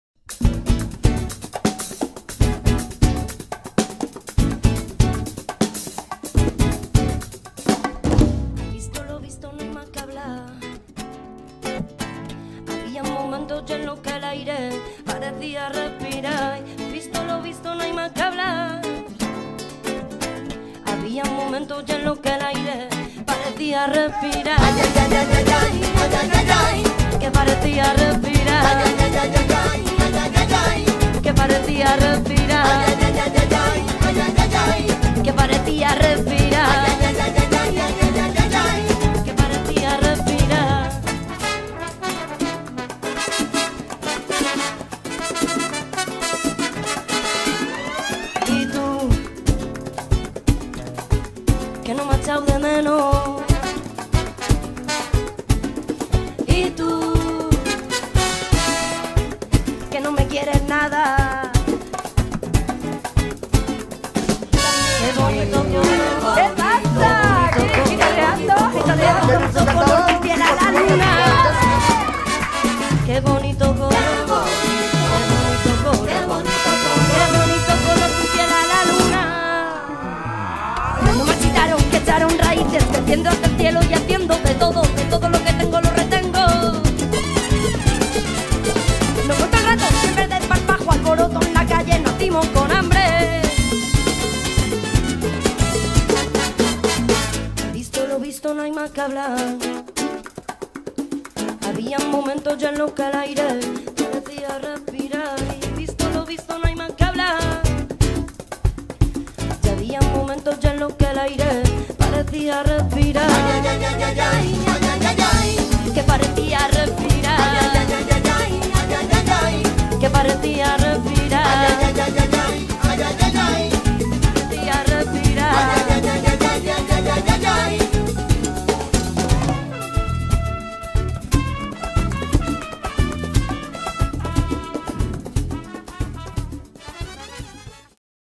Résumé Setmana de la Rumba 2016
résumé en images du festival Setmana de la Rumba Catalana 2016 - concerts : Tato et Peret Reyes, Maria Cambray, Anton, Bily de figueras, Rumbamazigha, Achilifunk, Sabor de Gracia, expositions avec Gabi Jimenez, els colors de la rumba, gitanos como tu, projet orchestre à l'école